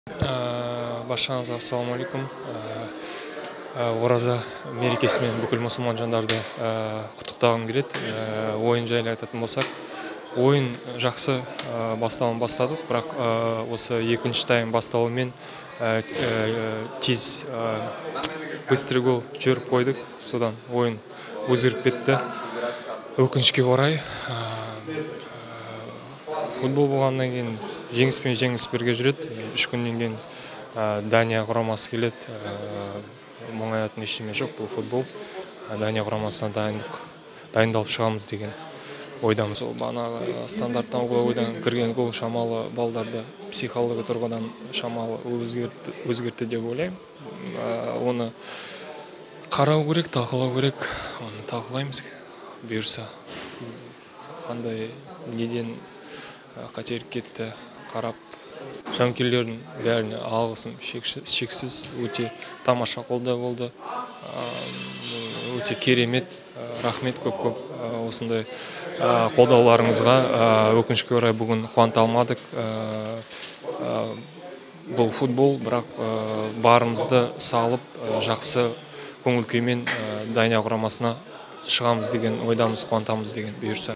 Өкінішке қарай бүгін қуанта алмадық | Айымбетов | Қазақстан – Словения
Қазақстан ұлттық құрамасының шабуылшысы Абат Айымбетов Словениямен болған матч туралы